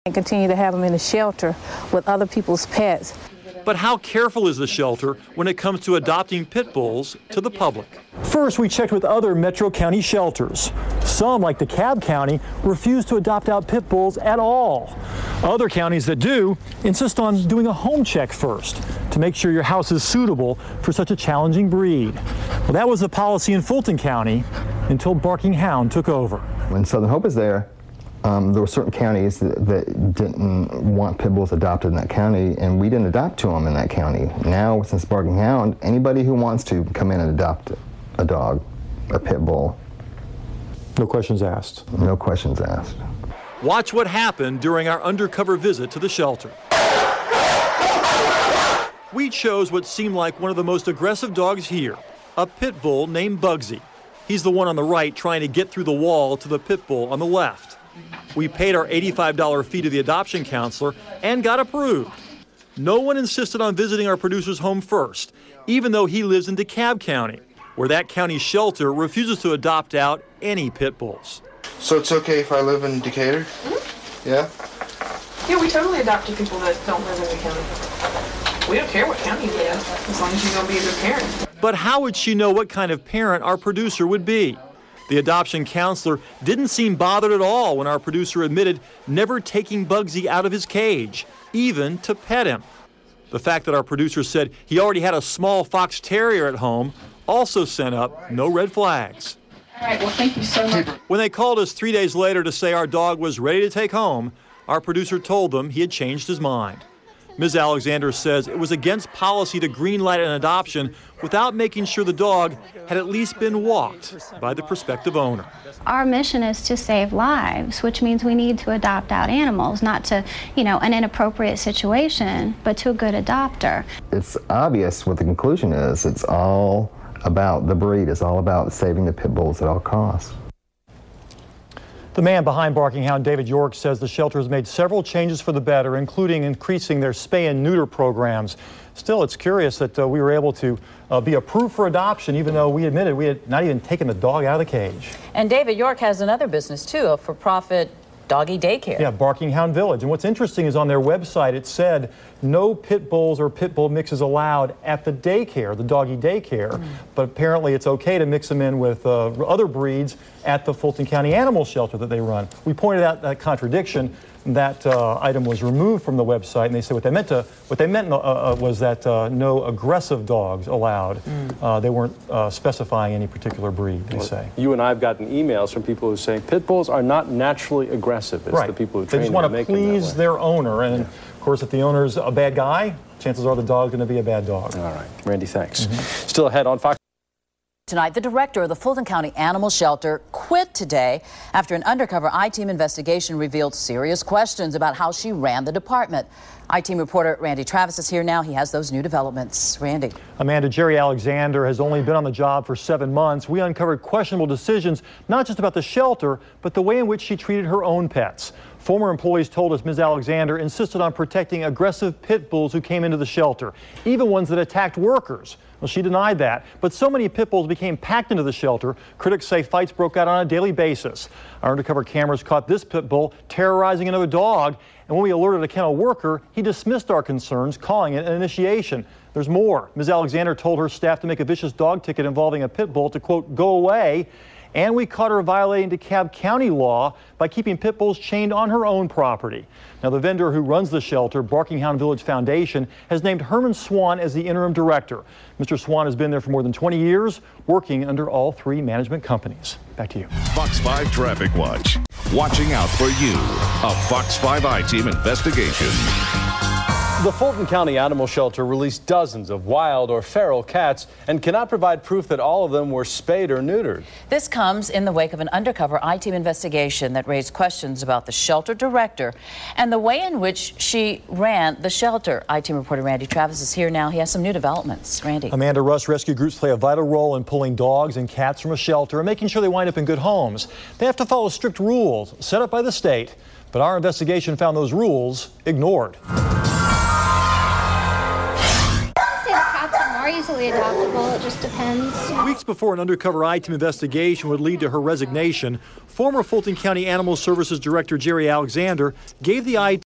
I Team Investigates Barking Hound Village Foundation pt3 | Fulton County Animal Services
Fox 5 investigates Barking Hound Village Foundation Director Gere Alexander and Dave York.